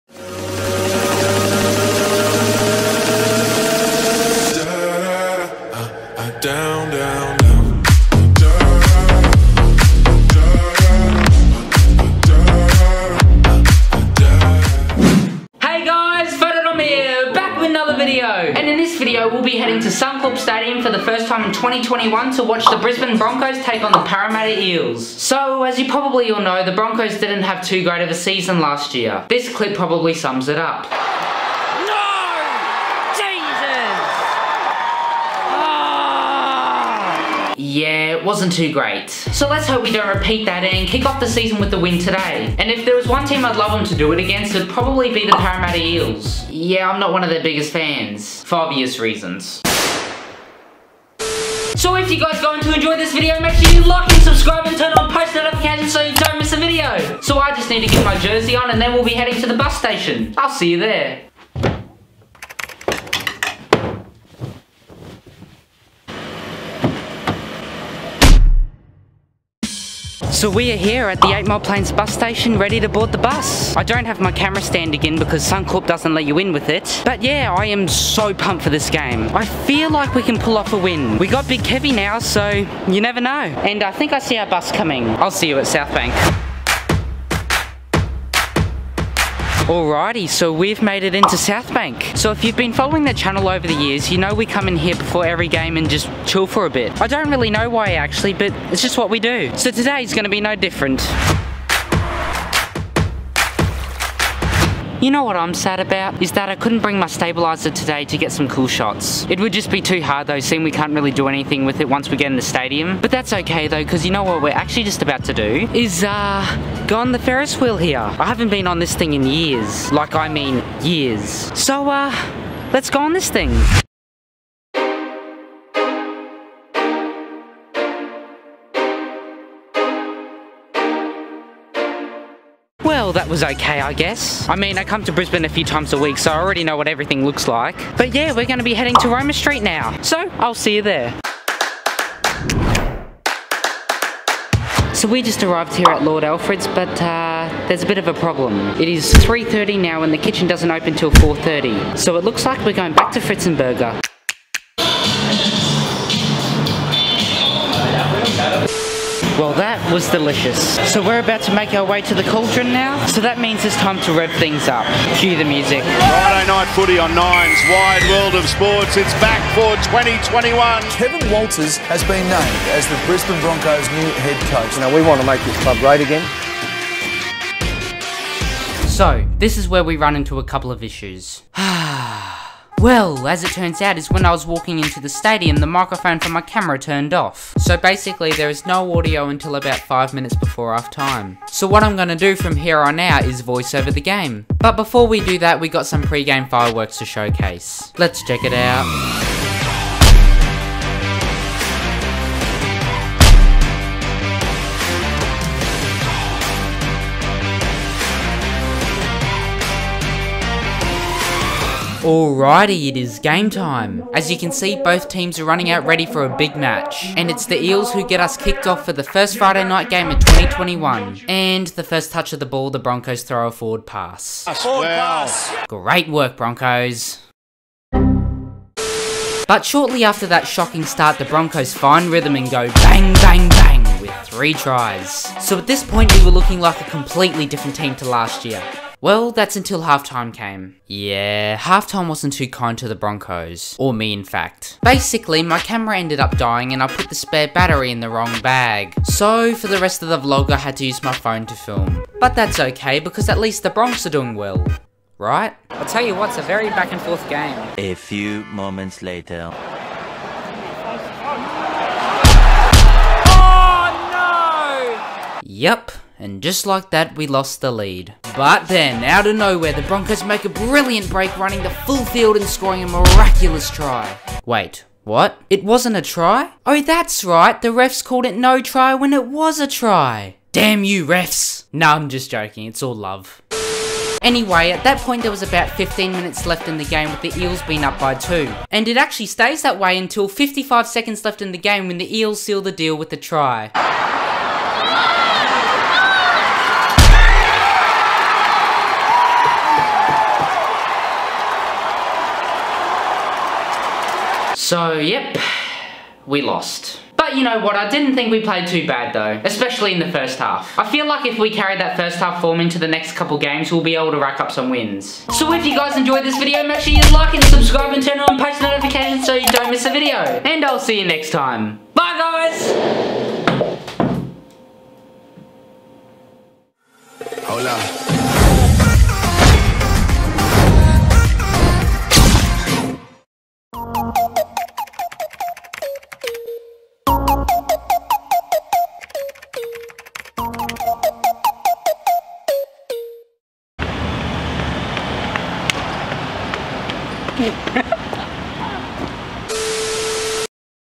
This Game Was INTENSE! |Broncos VS Eels 2021| (Vlog 40)
 Tell me if you enjoyed the video or if you have any feedback
 Instagram is (@PhotoDom.YT) and (@Dominic.Nipperess)
 Twitter is (@DominicNippy)
 New video every 2-4 Weeks (With the possibility of been longer)
In this video we head to Suncorp Stadium to watch the Brisbane Broncos take on the Paramatta Eels for the 1st Friday night game for 2021!